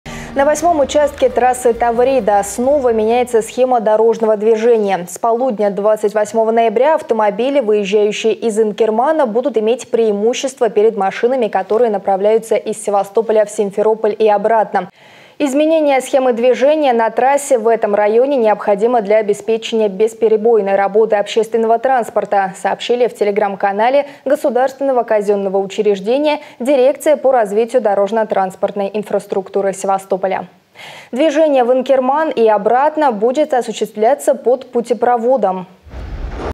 На 8-м участке трассы «Таврида» снова меняется схема дорожного движения
На 8-м участке трассы «Таврида» снова меняется схема дорожного движения. С полудня 28 ноября автомобили, выезжающие из Инкермана, будут иметь преимущество перед машинами, которые направляются из Севастополя в Симферополь и обратно.